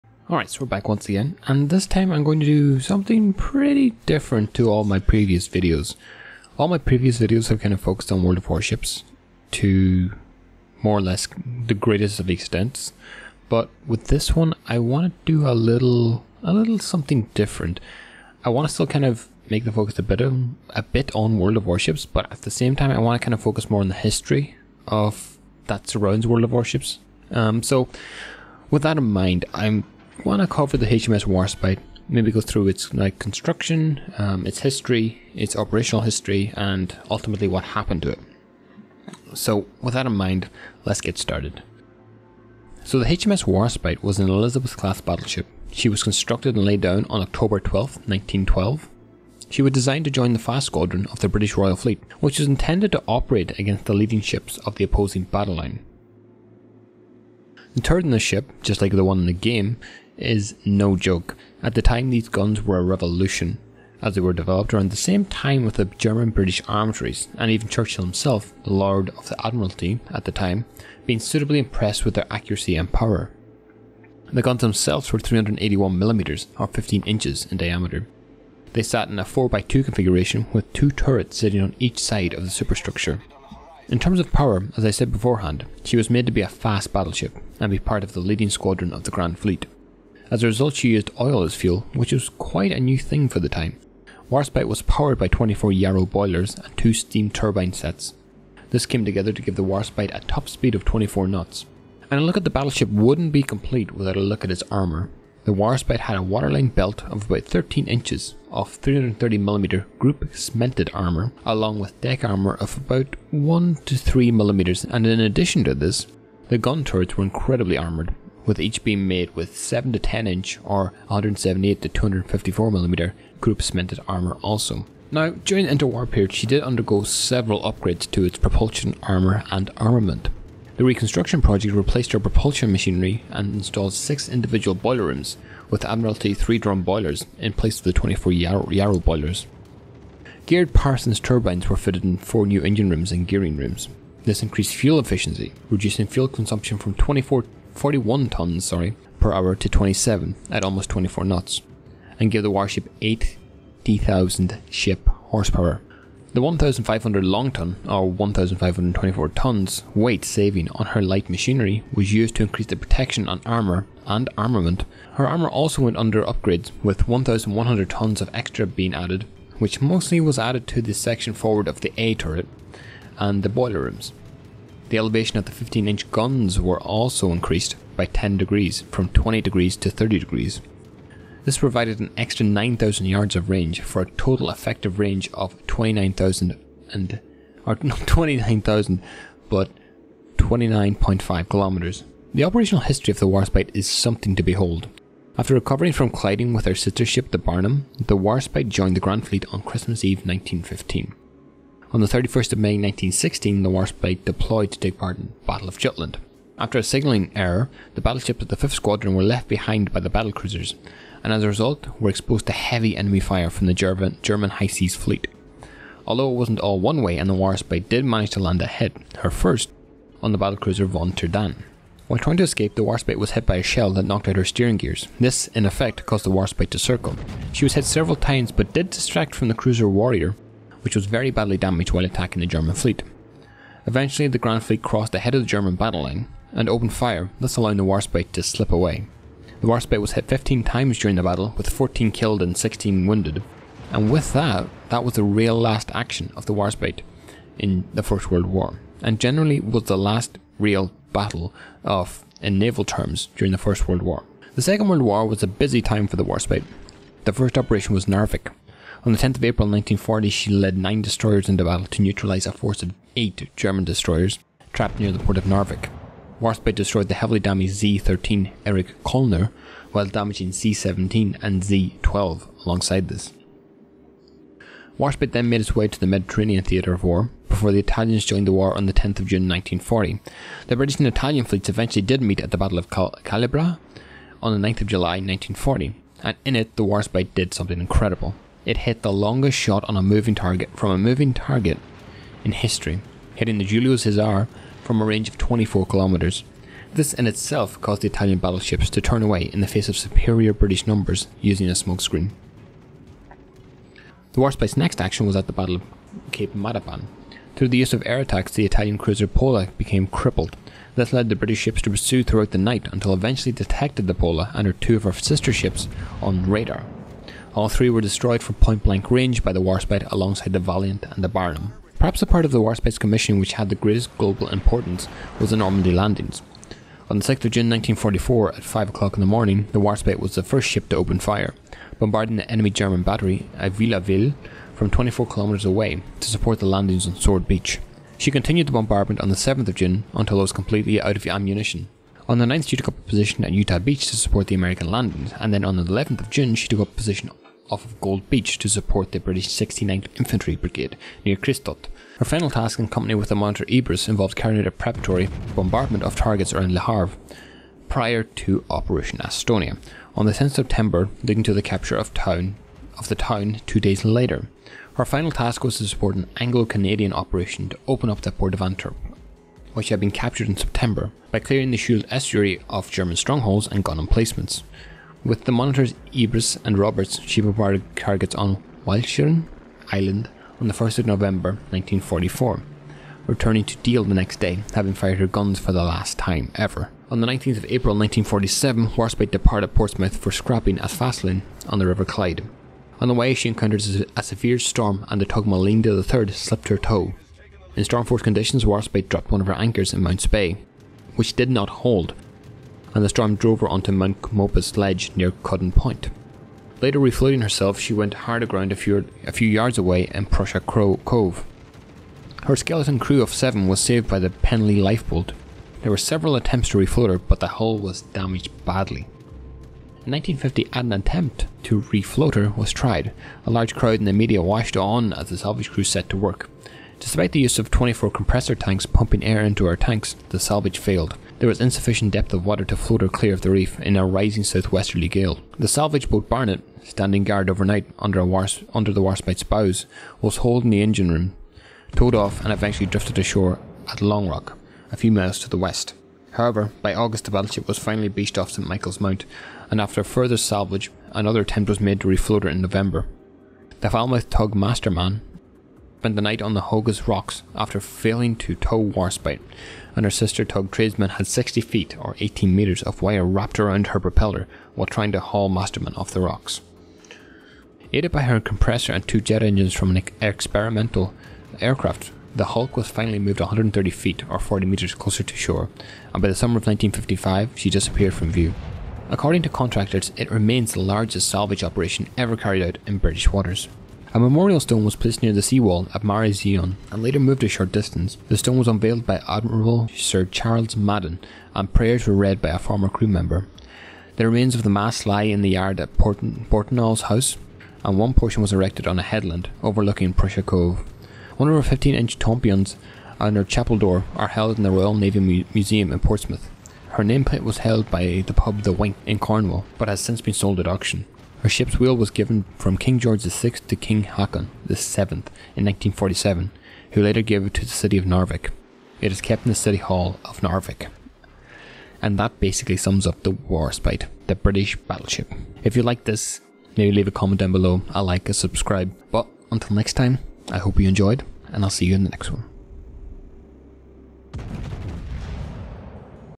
HMS Warspite -The Most Decorated Royal Navy Battleship - A History
HMS Warspite was an elizabeth class battleships, commissioned in 1912.
In this video I break down the life of the HMS Warspite and go over some of
the HMS Warspites operations and engagements. I hope you enjoy this overview of the HMS Warspite and until next time!